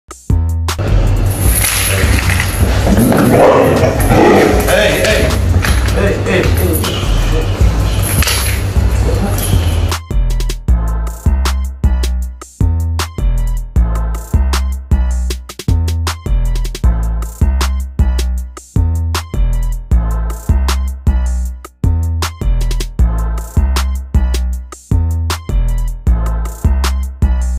Expensive lifestyle of Arab zoo owner Humaid Abdulla playing with wild pets
Humaid Abdulla Albuqaish is one of the youngest rich kid in the world, here is a video into his lifestyle in his jungle and around the world. All thanks to him for allowing me to use his post